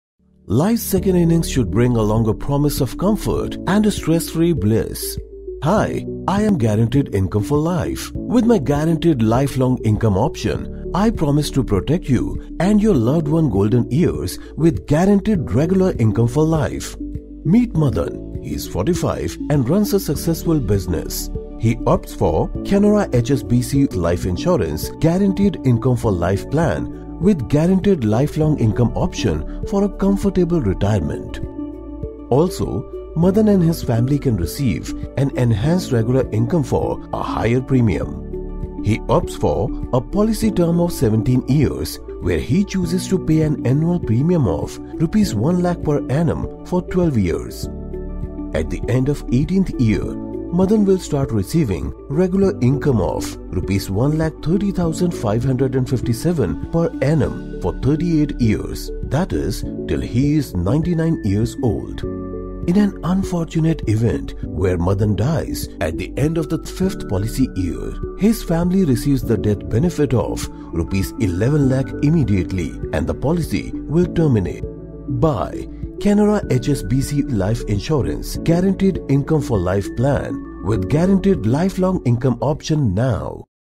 GI4L | Canara HSBC Life Insurance
Under the Guaranteed Income 4 Life plan, you can opt for 'Guaranteed life long income', which makes life easier and comfortable through the golden years of life after retirement. Watch the video to know everything about the plan and make the right decision.